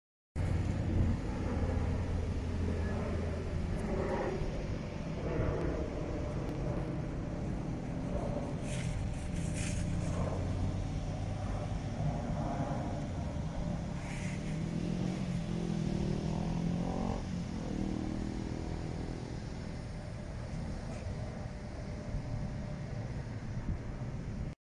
Airbus A400 M Militärtransporter im Tiefflug über Uniklinik Frankenthal
Heute flog ein Airbus A400 M tief über Frankenthal